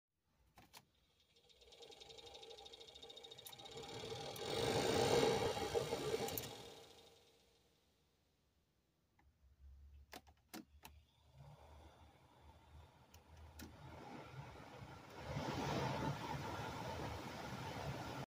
Car AC/Heater fan clicking noise comparison (blower motor bad bearing)
This video is meant to help with diagnostics; this is an example of a bad bearing. For instructions on the procedure search youtube for your make+model+year and "blower motor replacement" or "installation", and you'll probably find a detailed guide.

this example is a toyota corolla